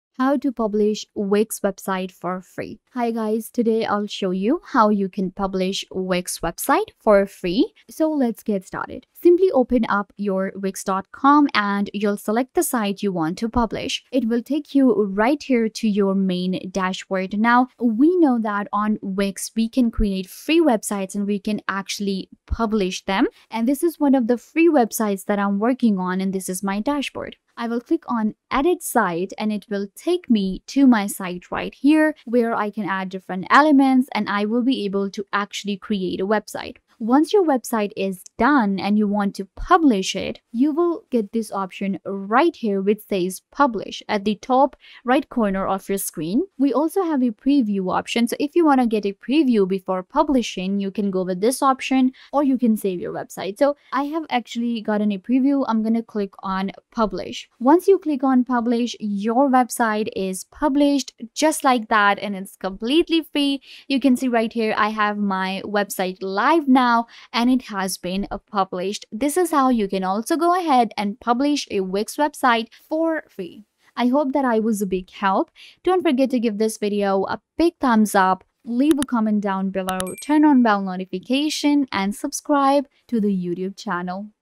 How To Publish Wix Website For FREE 2026 (UPDATED WAY)
In this video I show you how you can publish a wix website for free. We'll cover everything about publishing the website so this short video will give you alot of value.